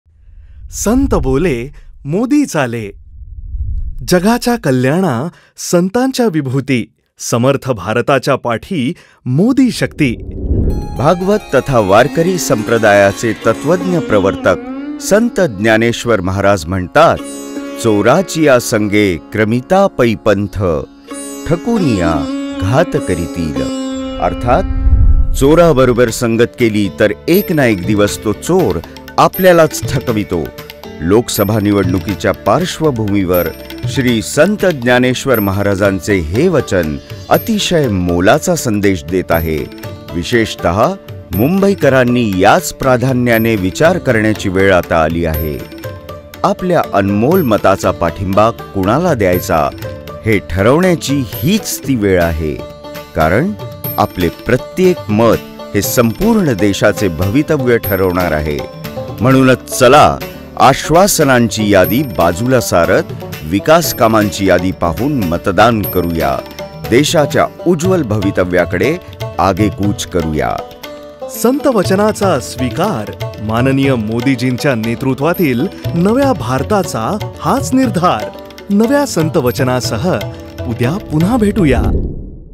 आश्वासन नव्हे, विकासकामे पाहून मतदान करुया, भारताचे भवितव्य उज्ज्वल करुया! | DCM Devendra Fadnavis
|| संत बोले मोदी चाले ||
आश्वासन नव्हे, विकासकामांची यादी पाहून मतदान करुया, भारताचे भवितव्य उज्ज्वल करुया!